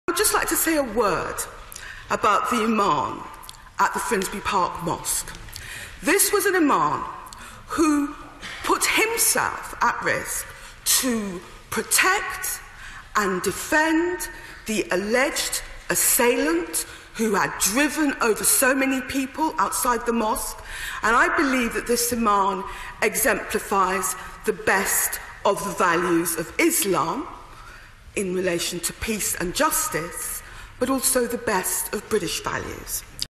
Diane in Parliament | "This Imam exemplifies the best of British values"
Diane Abbott MP praising the Imam Mohammed Mahmoud who became a nation's hero after shielding the terror suspect until police arrived on June 19th (22-06-17)